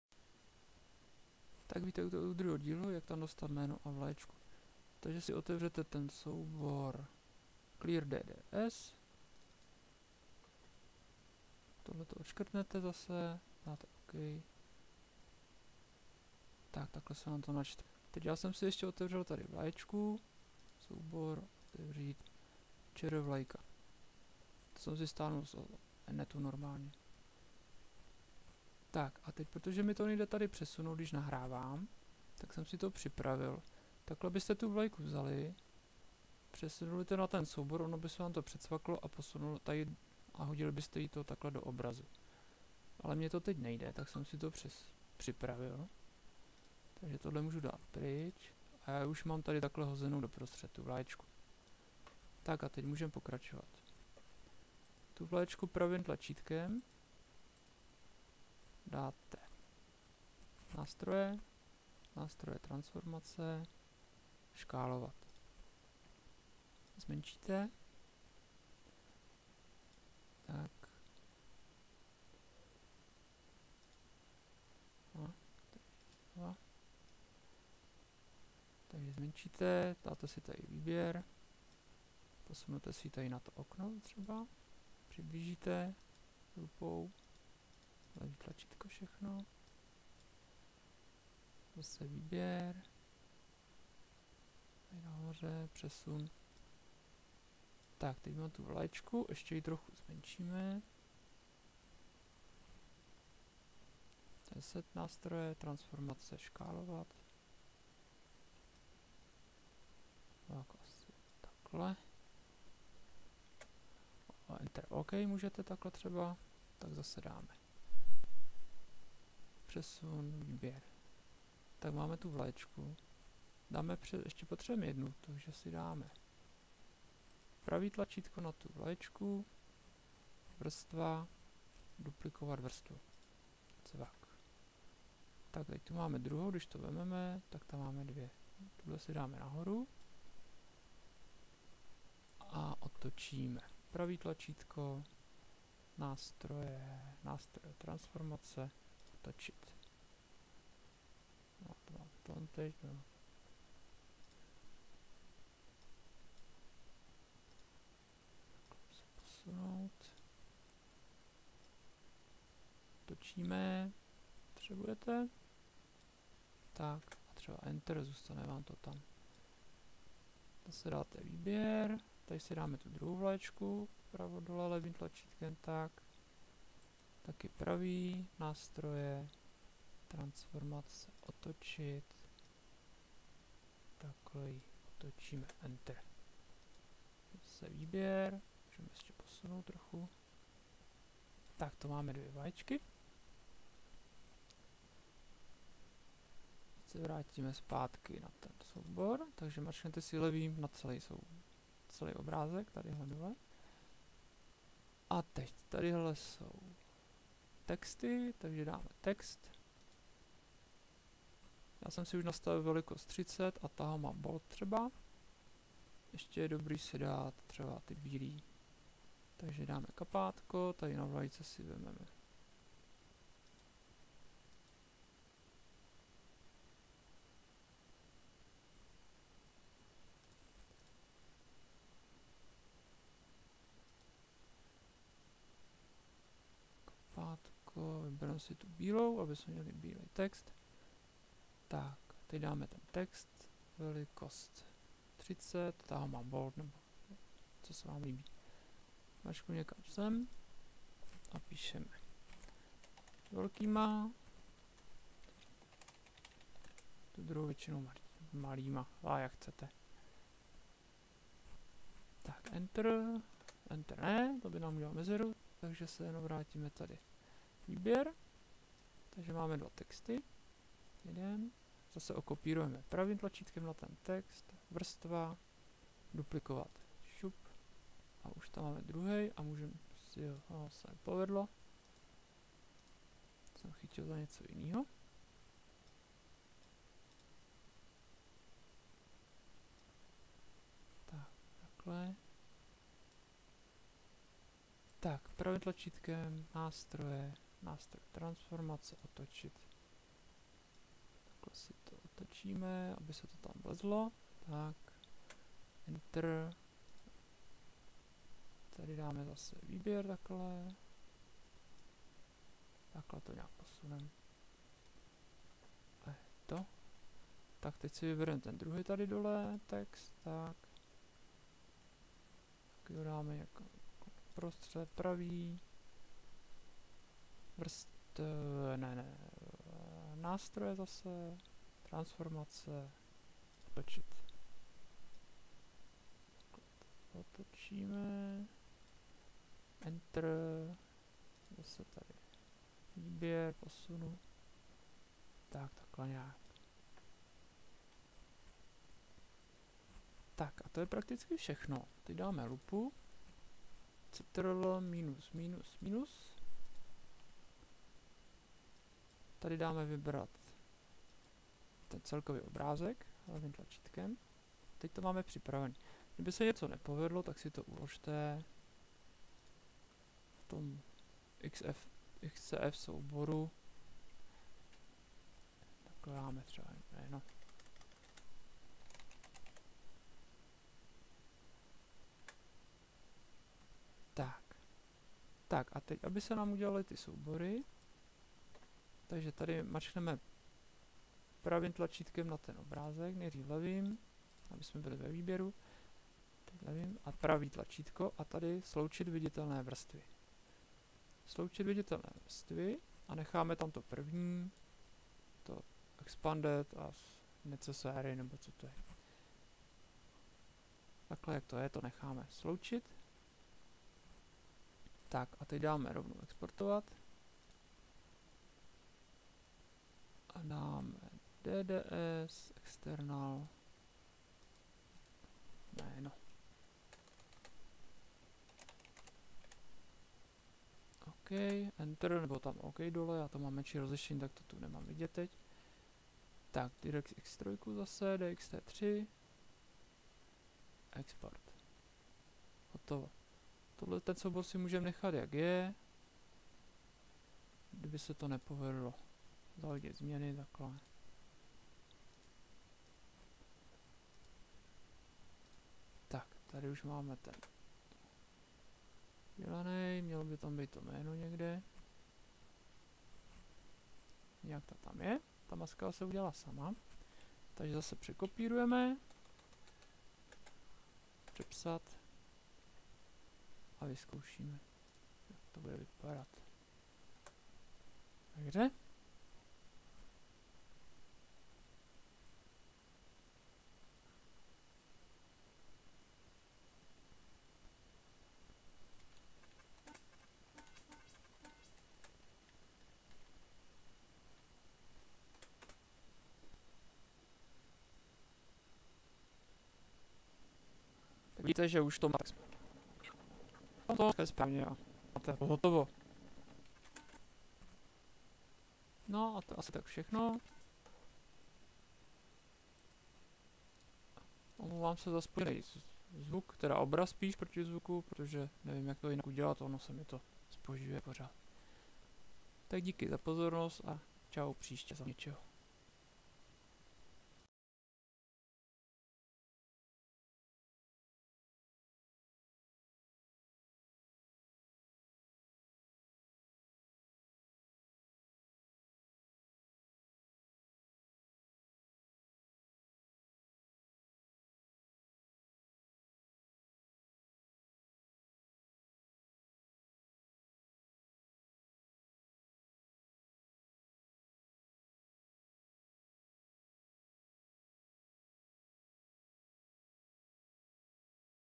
RBR dds car file GIMP 2.10 you new name and flag
RBR dds car file GIMP 2.10.28 you new name and flag.
Jak si dát v Gimpu jmenovku na okno s vlajkou v DDS textuře do RBR .
POZOR: obraz se mi bohužel zpožďuje za zvukem. Tak s tím počítejte :)